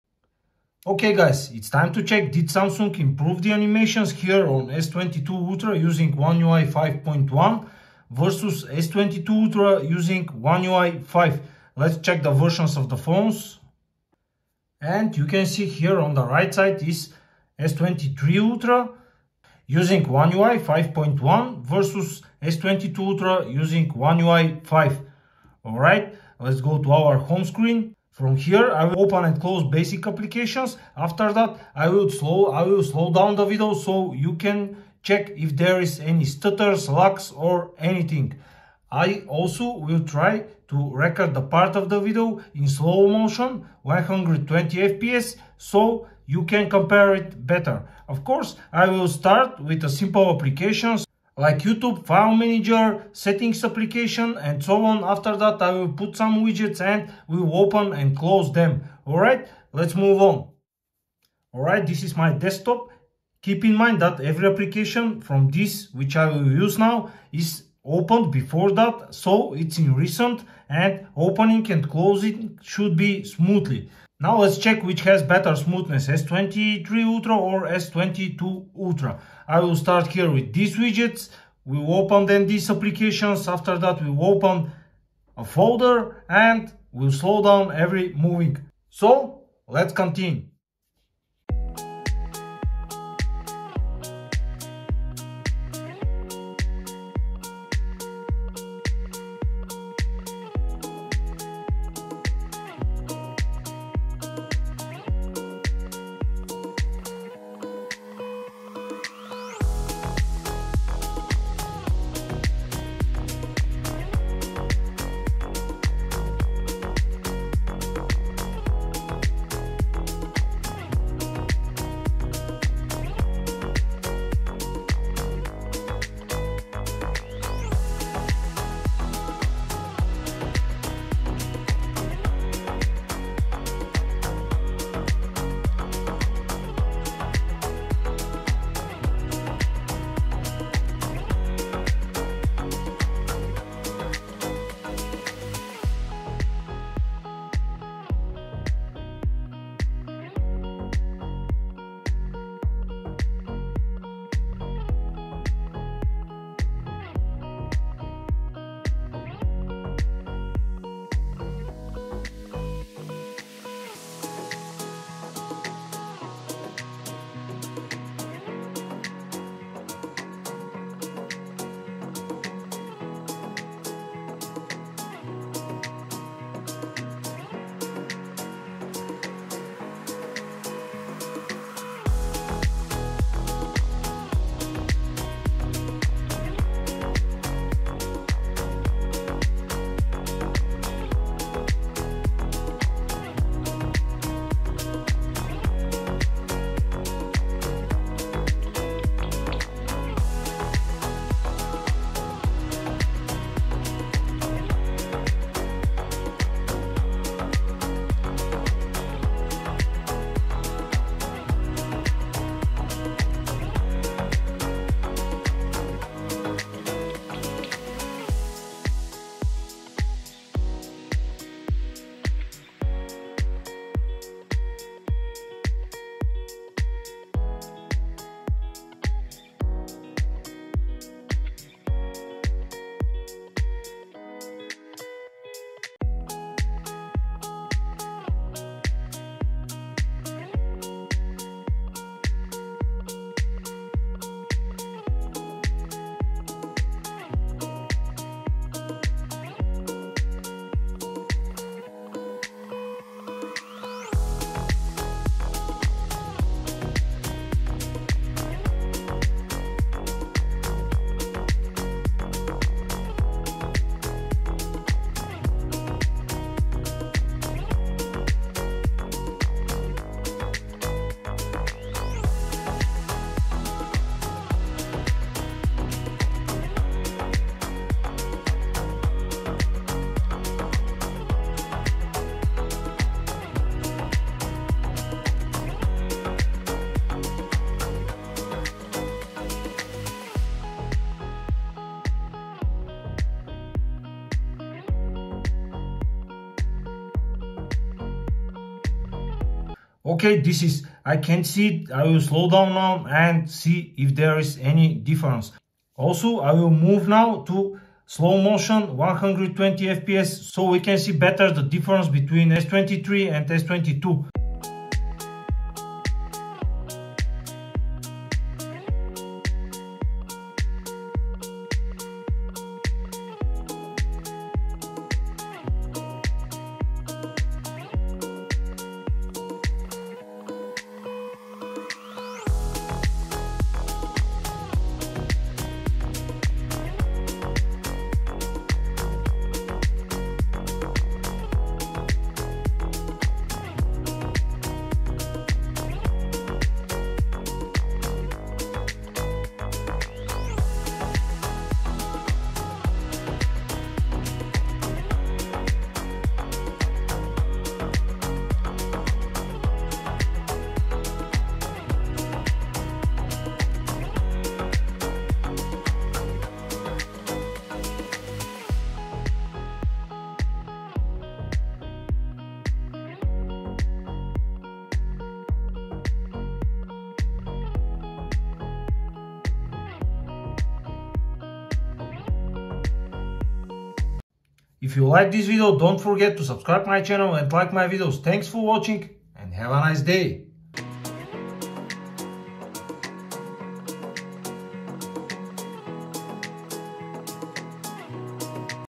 Samsung S23 Ultra vs S22 Ultra Animations Comparison - One Ui 5.1 vs 5.0
Slow-mo video comparison between Samsung Galaxy S23 Ultra & S22 Ultra. Animations comparison of One Ui 5.1 vs 5.0 is there any frame drops?